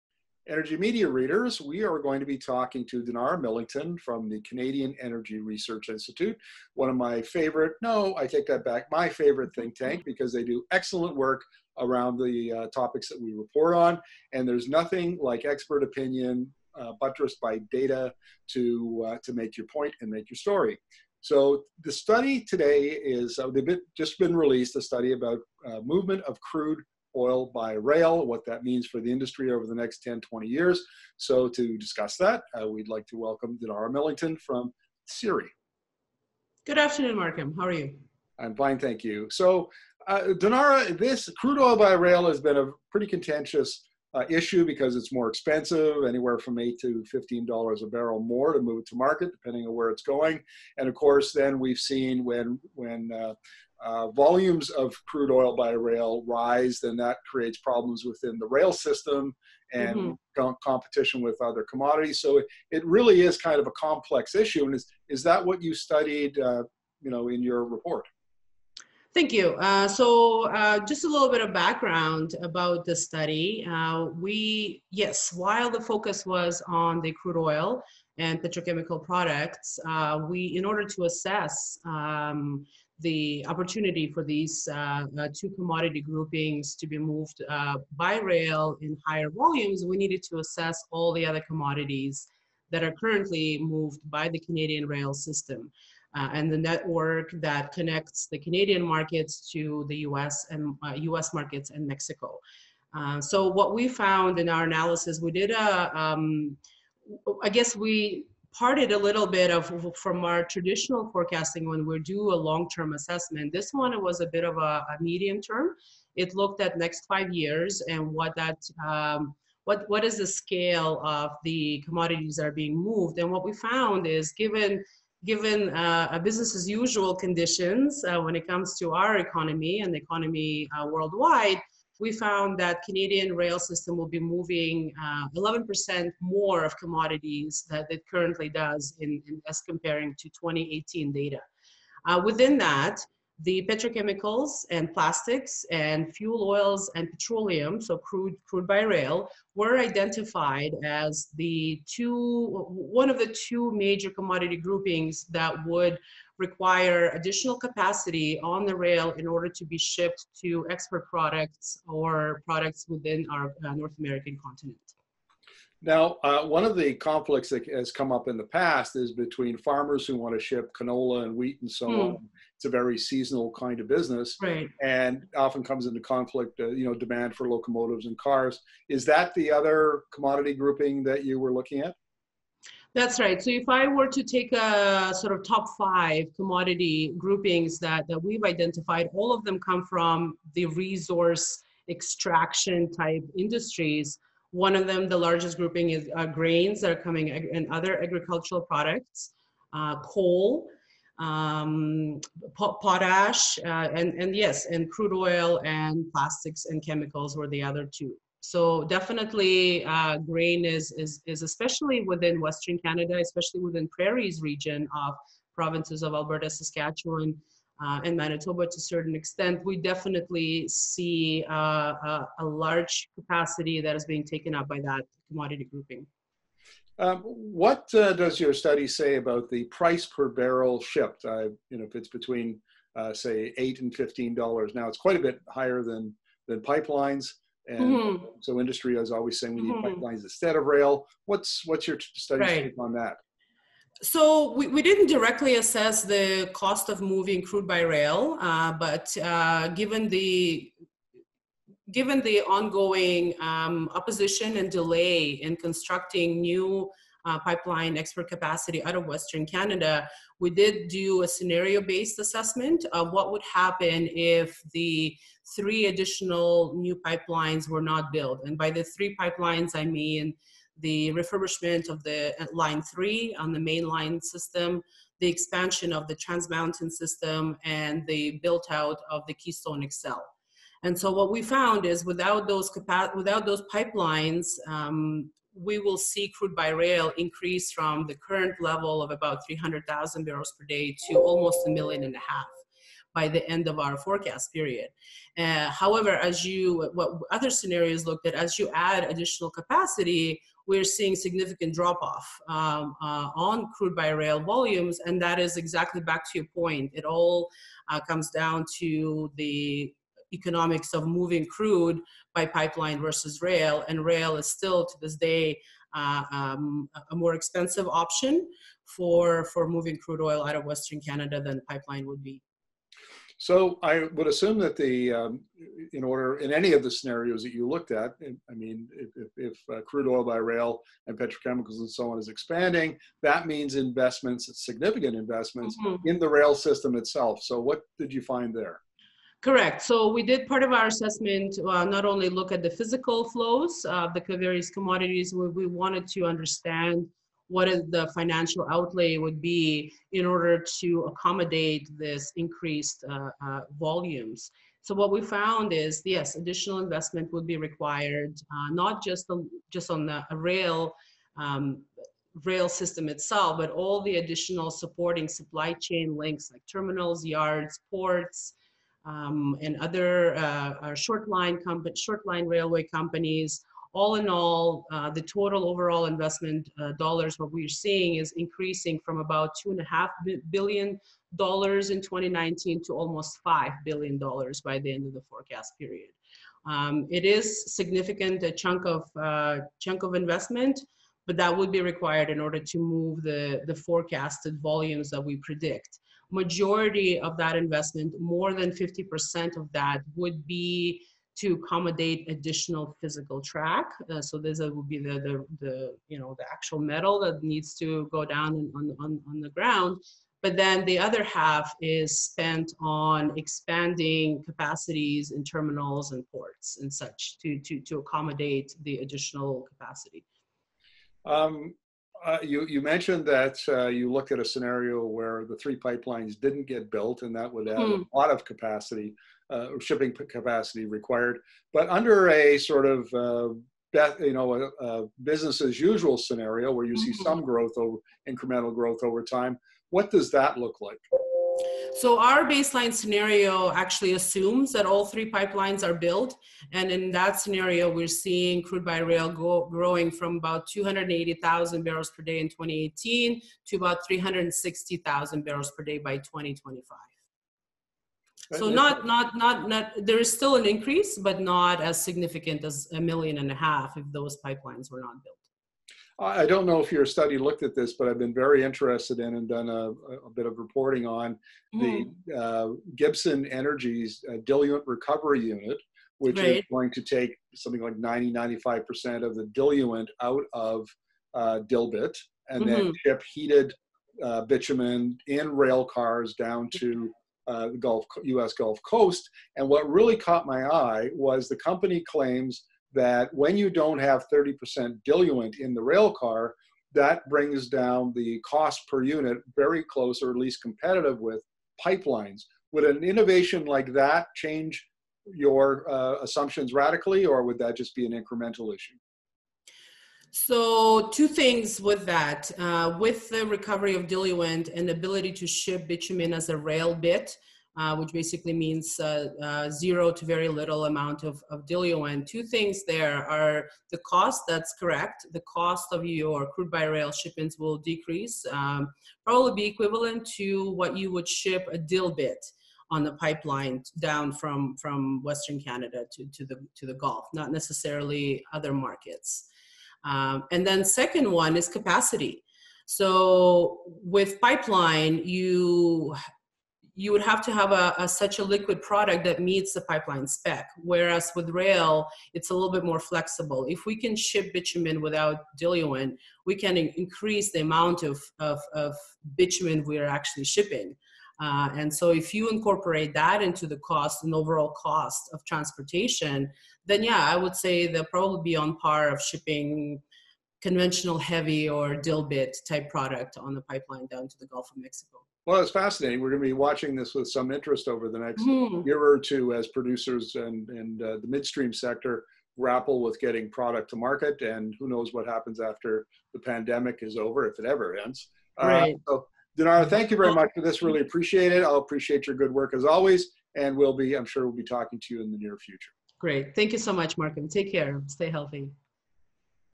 Shipping by rail and the Economic future of Canadian petrochemicals, petroleum fuels - CERI study
Ribbons of Steel: Ensuring an Economic Future for Petrochemicals and Petroleum Fuels

CERI has completed a detailed review of how rail impacts Canada’s crude and petrochemicals markets. One of the main objectives of this study was to determine the makeup of commodities currently transported by the rail system in Canada. This information, combined with a perspective of future growth or decline on a commodity by commodity basis, allowed CERI to establish a future view of the commodities that could be moving on the rail system in 2025, with a focus on petrochemicals and petroleum products, including crude oil.

The study also outlines the Canadian rail-based supply chain, including shippers, terminal operators, and ports, since the performance of the system can be affected by any stakeholders involved in the movement or handling of rail freight traffic. For the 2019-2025 period, on average, the fuel oils and crude petroleum and plastic and chemical products rail traffic will increase by 1.7 percent and 1.9 percent, respectively. In the case of all three oil pipelines currently under construction being completed, the crude and other fuels transported on rail would grow from 280,000 bpd in 2019 to 360,000 bpd by 2025. In a scenario where none of these pipelines are operational, the crude fuel volume is expected to reach almost 1.4 million bpd by 2025.

CERI’s investment model estimated that an additional investment of $339.2 million would be needed in the Alberta Industrial Heartland (AIH) to accommodate new facilities that will become operational in this period. As a result of additional investment, the AIH region will see further economic benefits from the spin-offs of this investment. The GDP impact is measured to be almost $6.7 billion over the six years, more than a 10-fold impact.

Rail system expected growth (includes all major sector shippers) results in need annual capital investment of $2.5 billion in 2019, growing to $4 billion in 2020 and reaching almost $5.3 billion by 2025. This represents a 26 percent growth in the short term from 2020 to 2025. It should be noted that these observations are based on a business as usual scenario and do not include the impact associated with the Covid-19 pandemic.